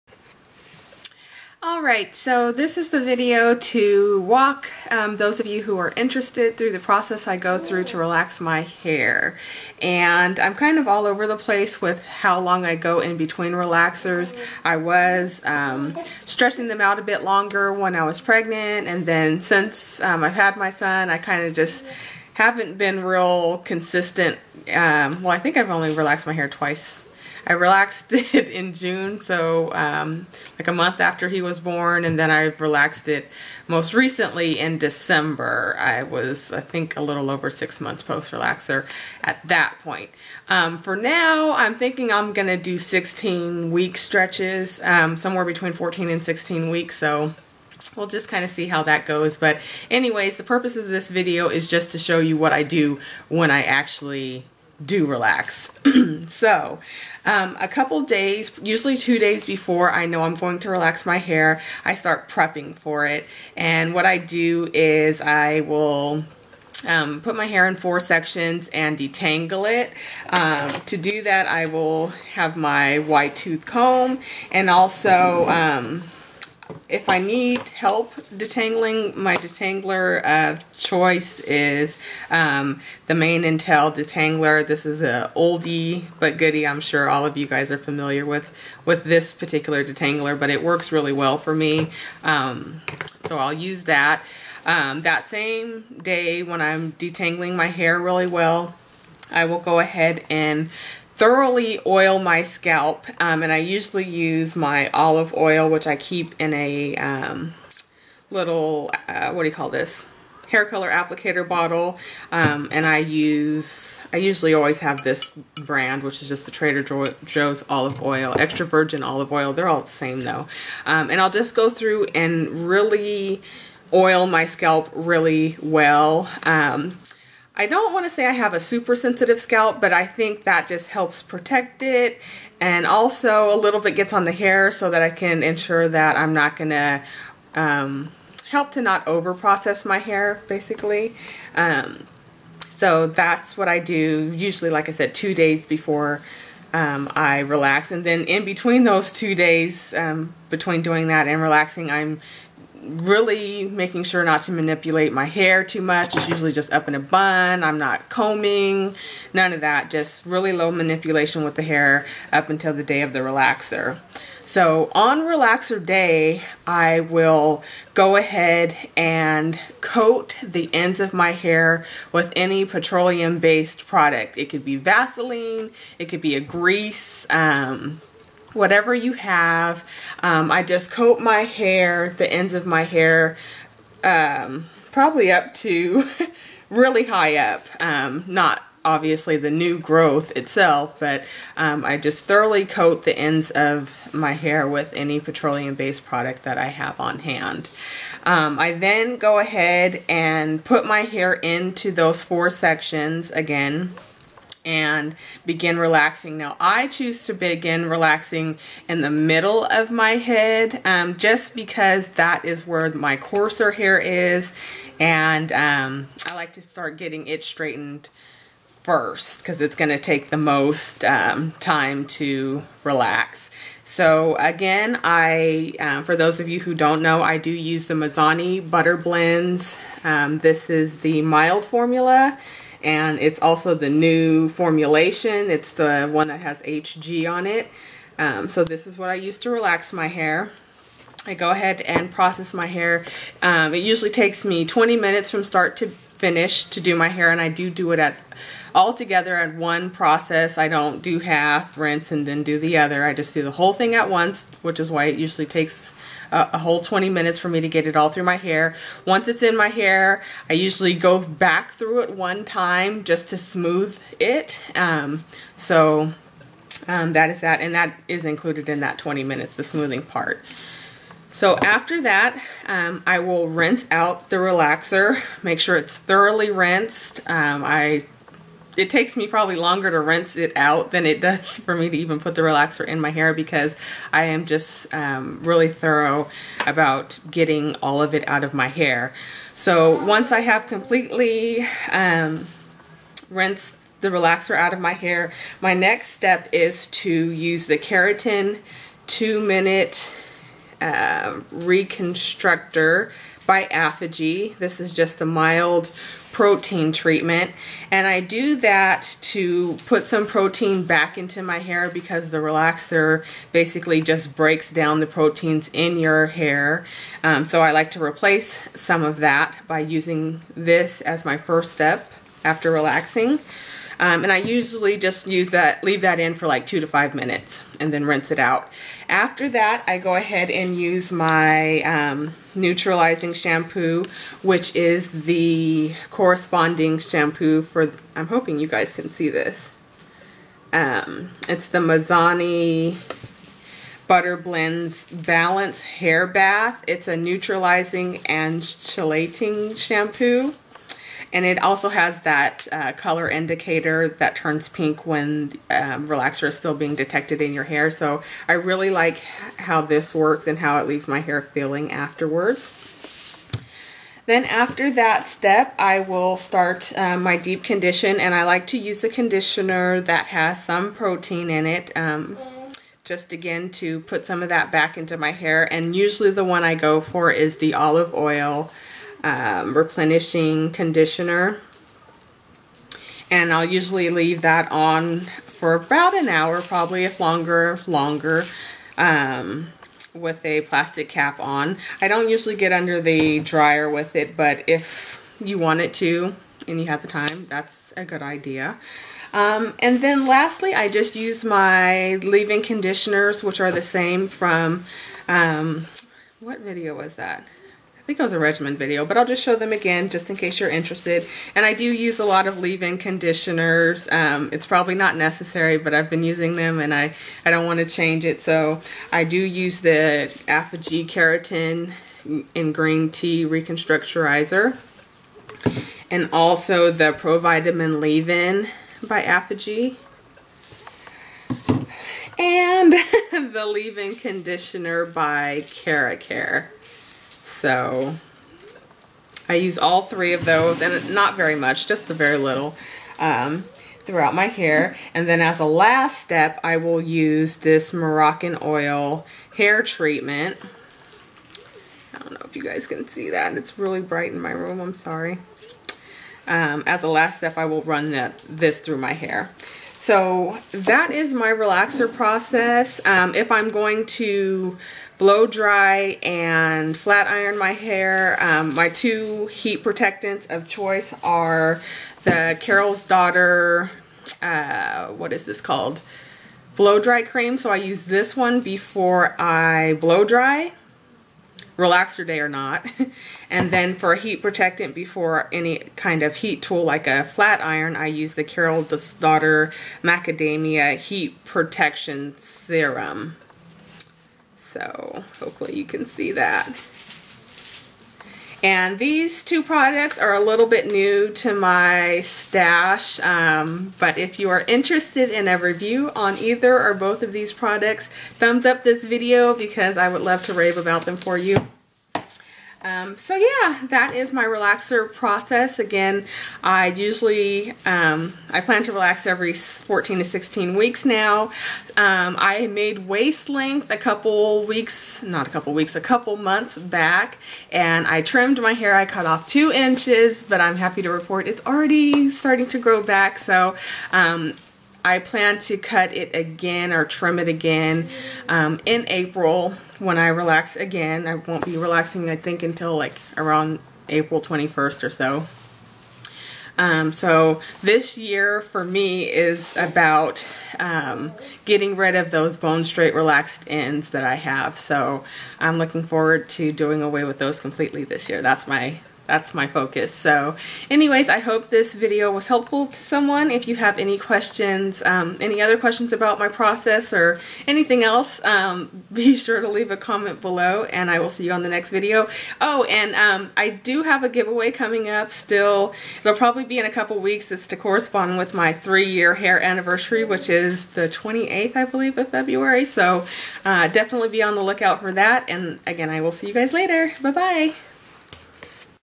How I Relax My Waist Length Hair
On this video I show the steps and products used when relaxing my hair